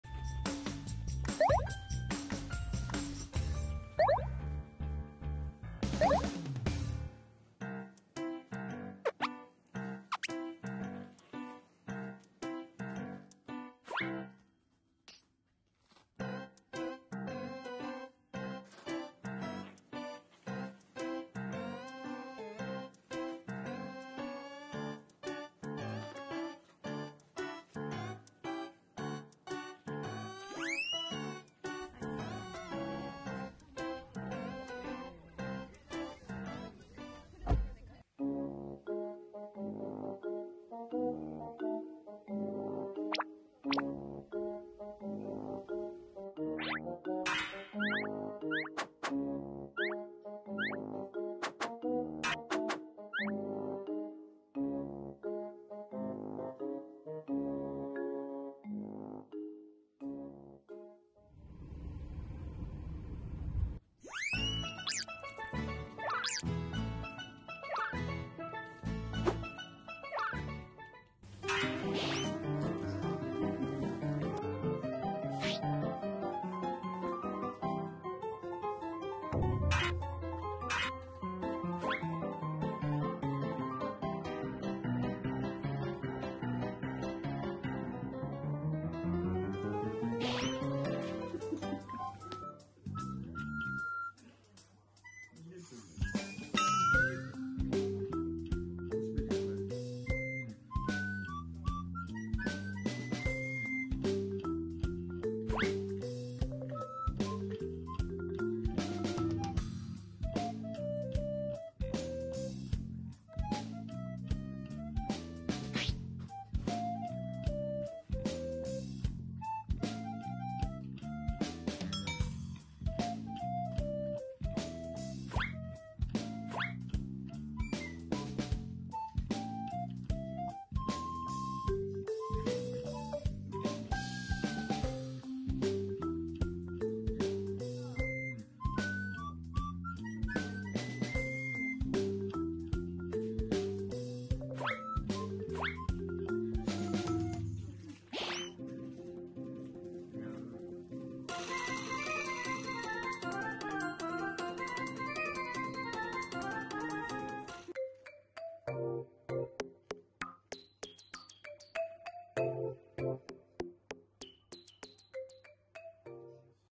学習してくるポメラニアン【251日目】
今回も苦戦をしいられました。そろっと近づいて様子を伺ってるところめっちゃ面白かったです。日中もゆっくりしていてくれて安心しました。違う作戦練らないといけないかもしれないです。